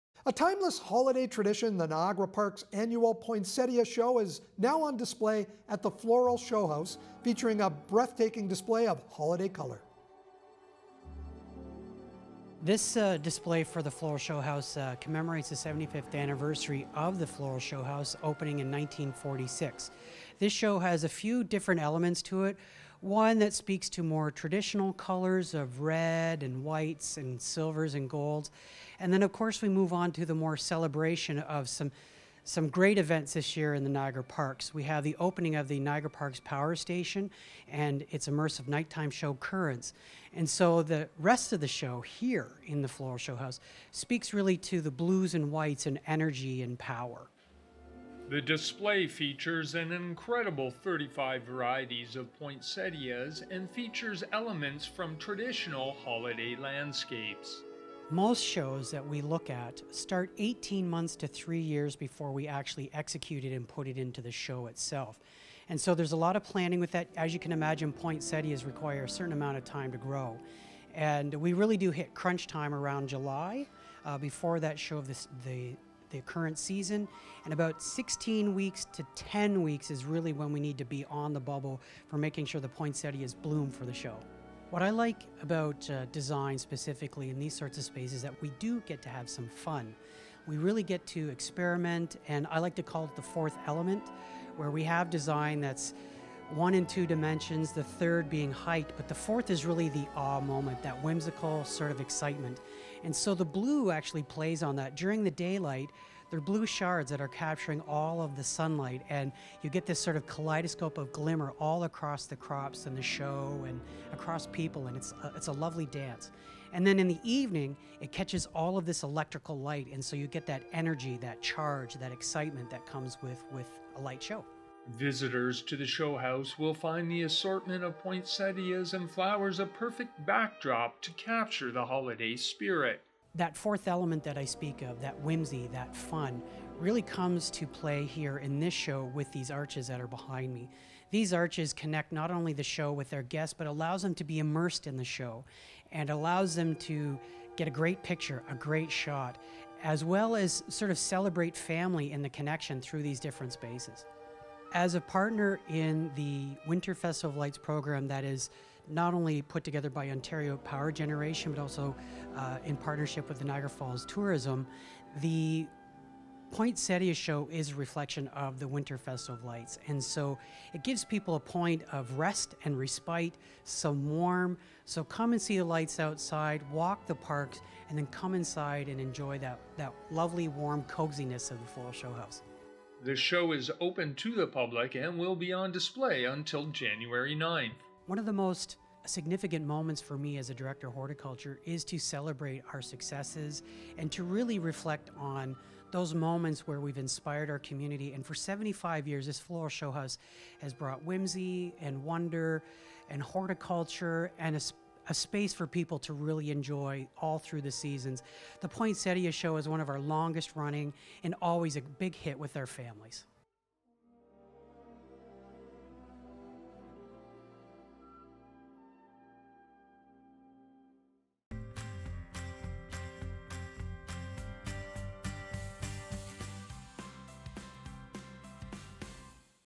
Niagara Parks Unveils This Year’s Annual Poinsettia Show at the Floral Showhouse
A timeless holiday tradition, the Annual Poinsettia Show, featuring breathtaking arrangements of poinsettias, cyclamen, caladiums and orchids, will open to the public on November 20 and run through to January 9, 2022, at the Niagara Parks Floral Showhouse.